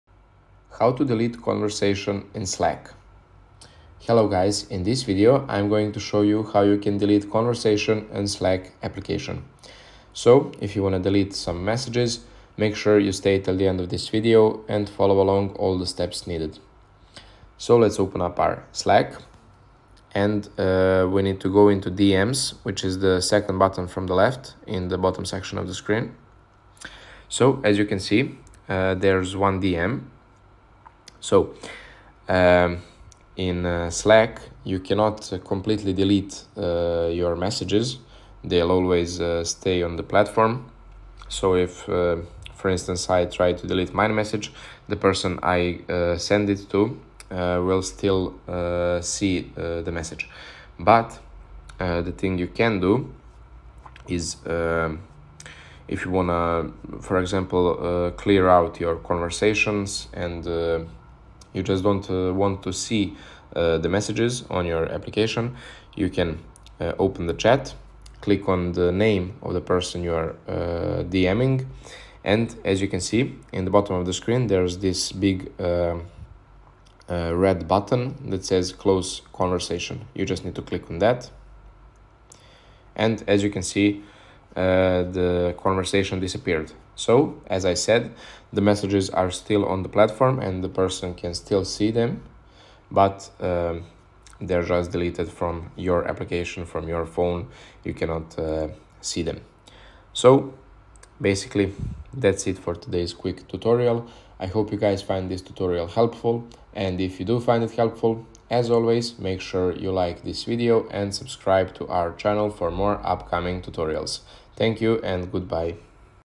How to Delete Conversation on Slack (Easiest Way)
Today we talk about How to Delete Conversation on Slack, so stay until the end of the video to see the full explanation.